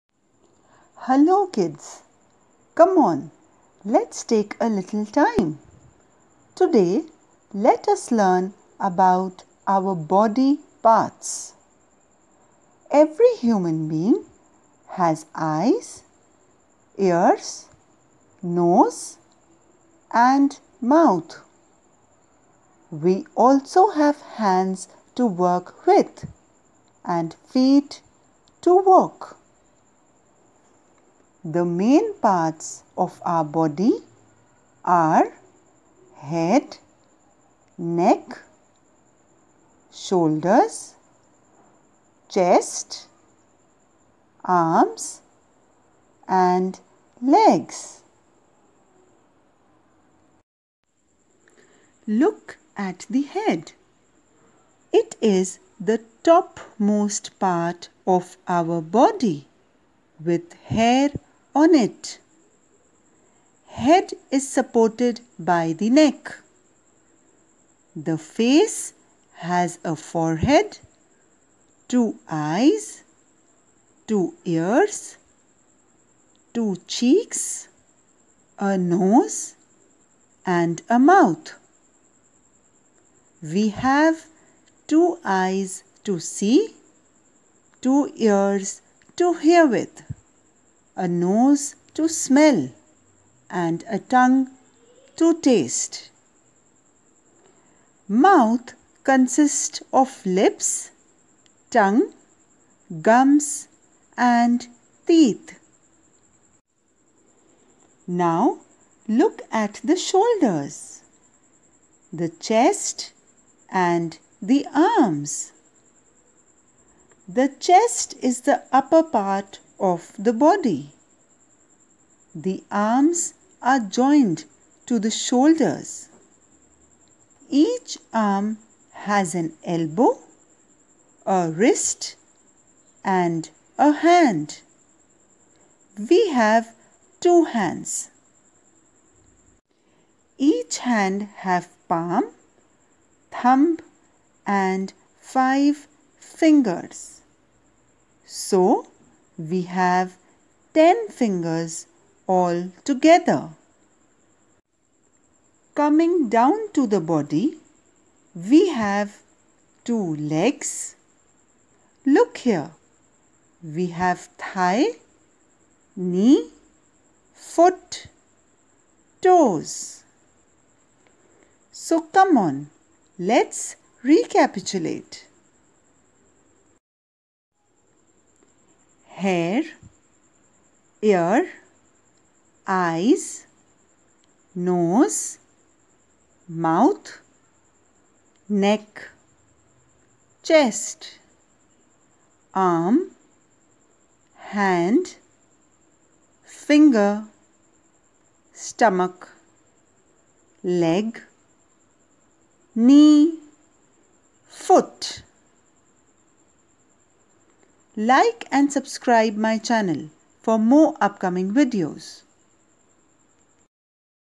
human body parts || parts of body for kids || body parts vocabulary
infotainment by af
parts of human body in english
an informative video for kid's to read n learn parts of body..
very easy way for toddlers to learn human body parts